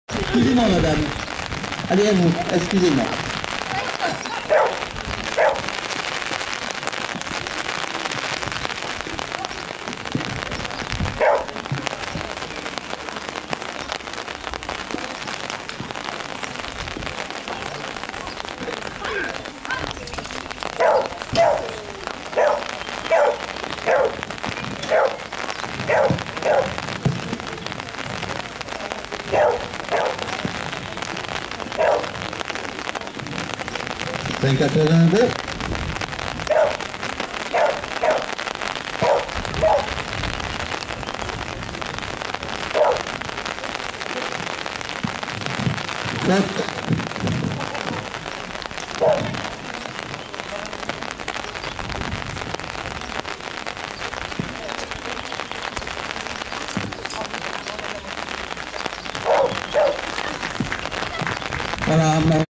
Eclipse 1er degré cat B Montceau juin 2012
Concours d'agility, Montceau les mines 3 juin 2012, 1er degré catégorie B